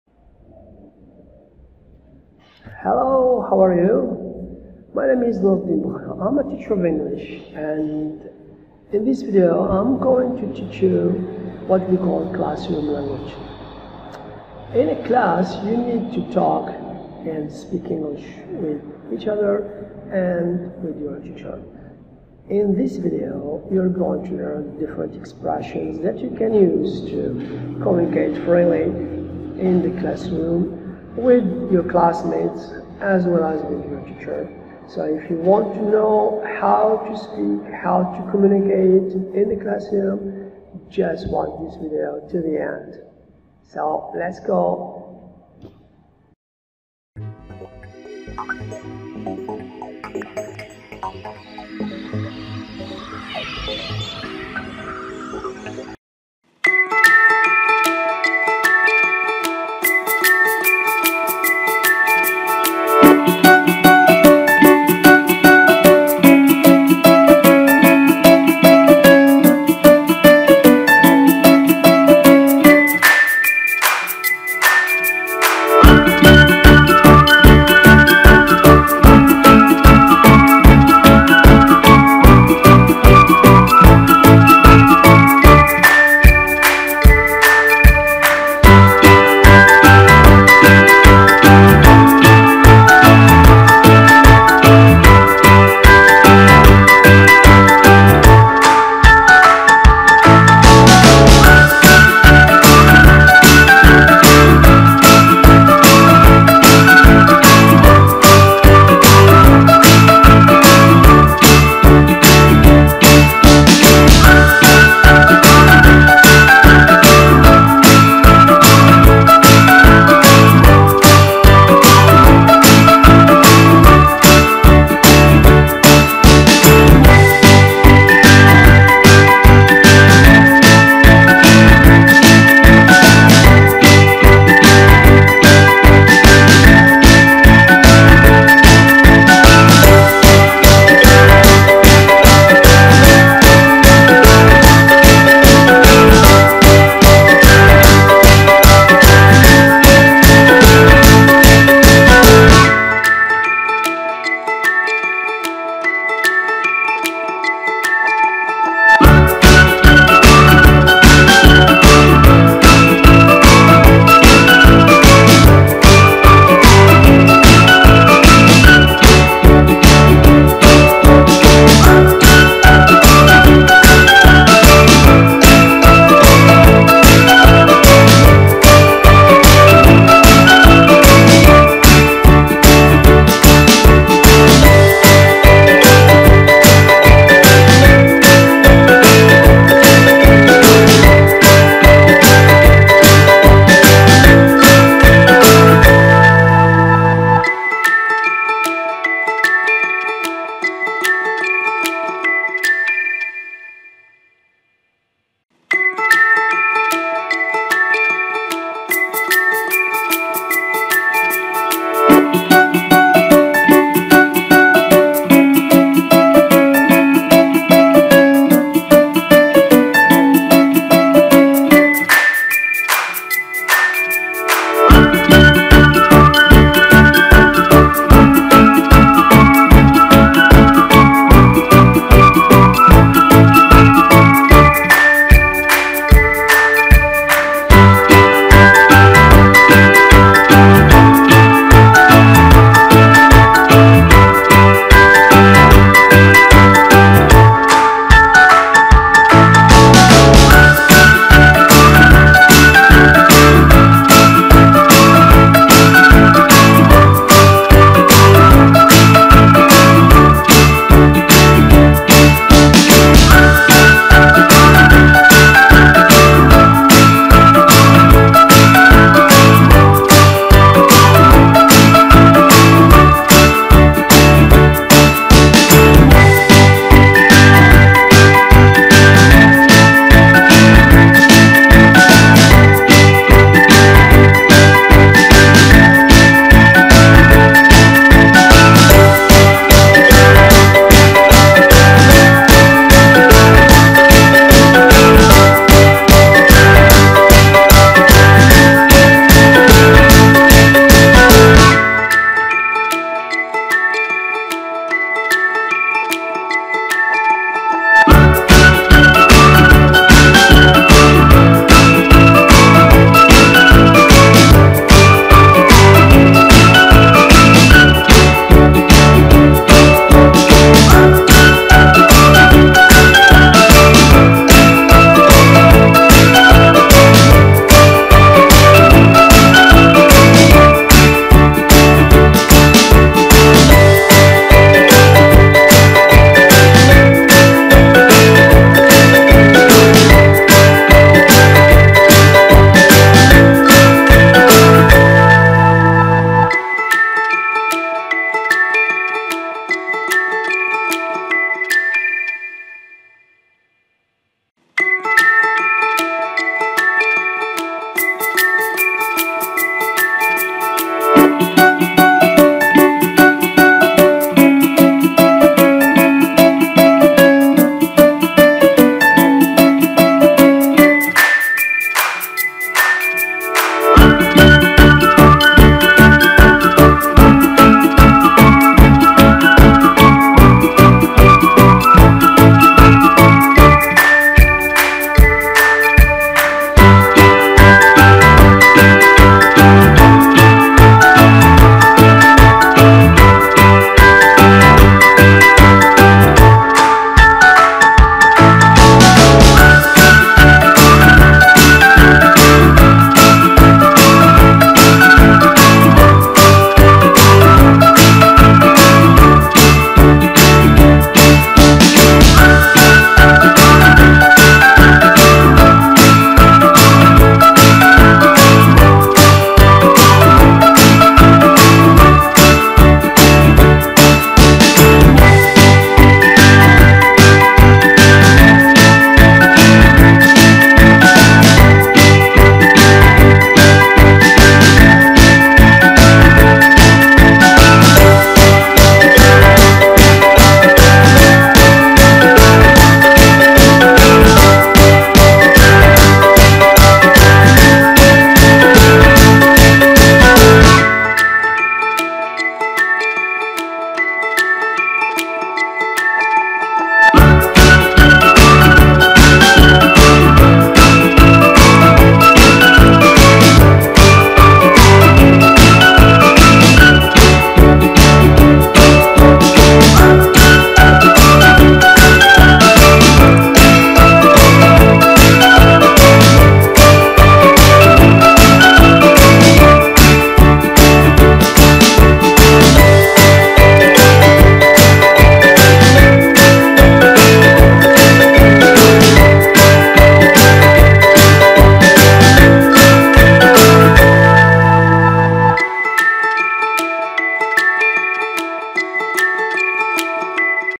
Classroom_language. لغة الفصل
In this video, you are going to learn different expressions and sentences to speak English and communicate in the classroom.
في هذا الفيديو ، ستتعلم التعبيرات والجمل المختلفة للتحدث باللغة الإنجليزية والتواصل في الفصل. #
Dans cette vidéo, vous allez apprendre différentes expressions et phrases pour parler anglais et communiquer en classe.